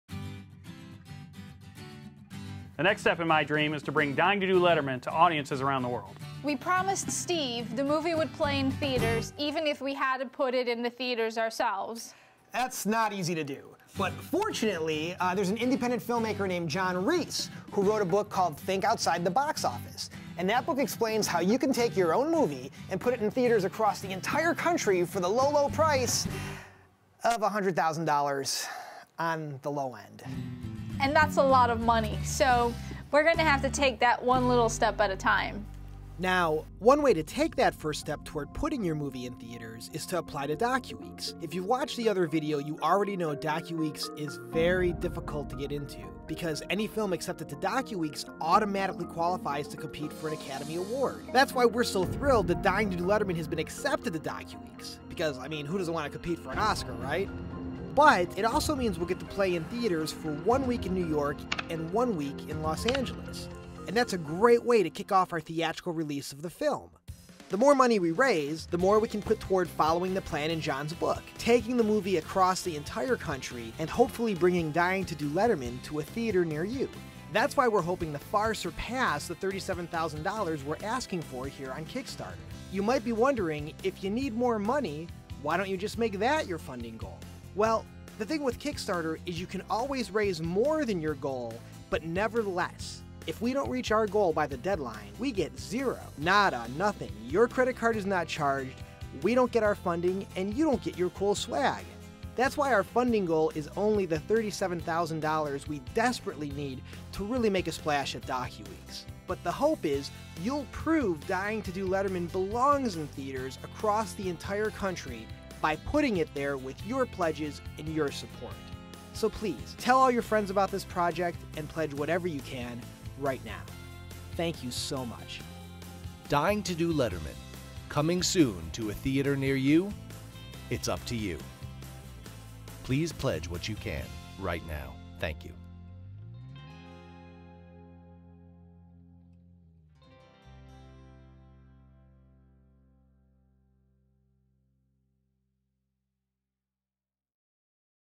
Thinking Outside the Box Office with Jon Reiss -- Putting a Movie in Theaters Yourself
This video explains why we'll use money we raise above our goal to bring Steve's story to a theater near you...thanks to a man named Jon Reiss and his book "Think Outside the Box Office."